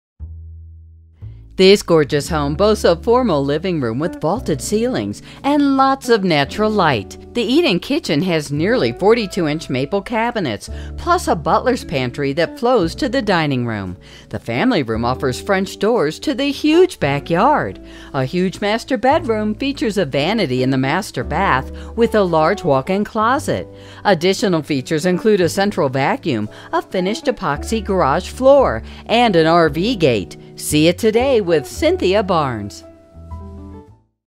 14335 W Lisbon Lane Surprise, AZ | ColdwellBankerHomes.com
14335 W Lisbon Lane, Surprise, AZ

Need more space? Check out this gorgeous, approximately 4,100 plus square foot home with six bedrooms and three and one-half baths on an extra-large lot featuring a gazebo and fruit trees. The formal living room has vaulted ceilings with lots of natural light. The kitchen has nearly 42-inch maple ca